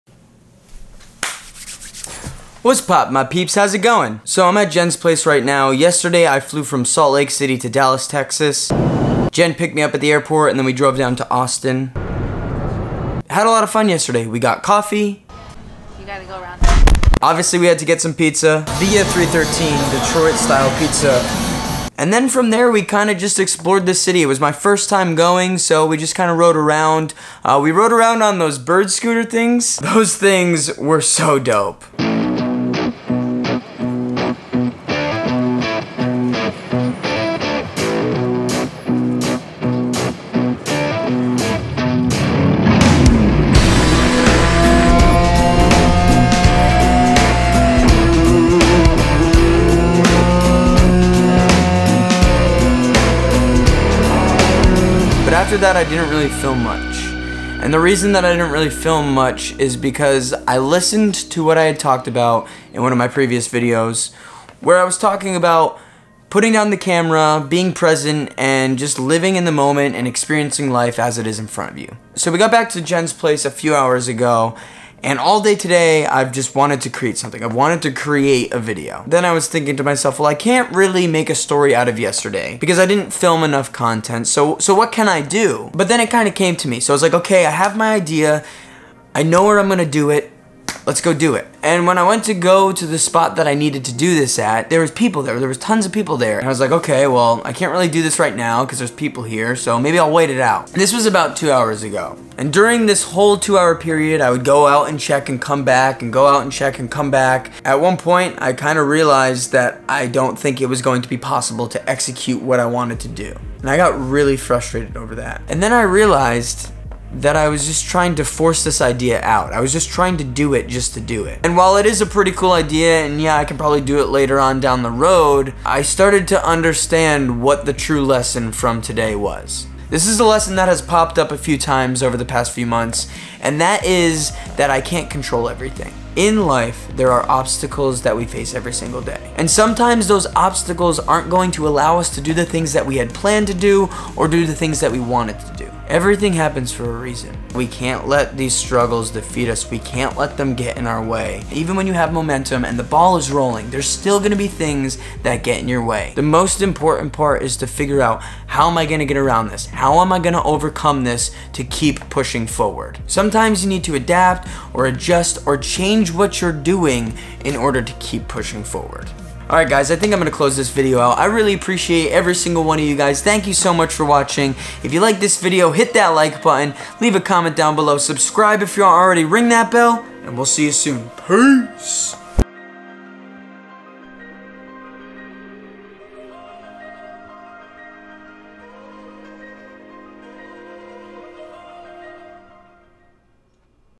The obstacles we face everyday.
The obstacles we face everyday. Sometimes there's going to be things that get in your way. In order to work around or overcome these obstacles, we need to adapt, and adjust to the situation, so we can keep pushing forward. This video is a talk about the obstacles we face everyday, as well as a little Austin recap vlog.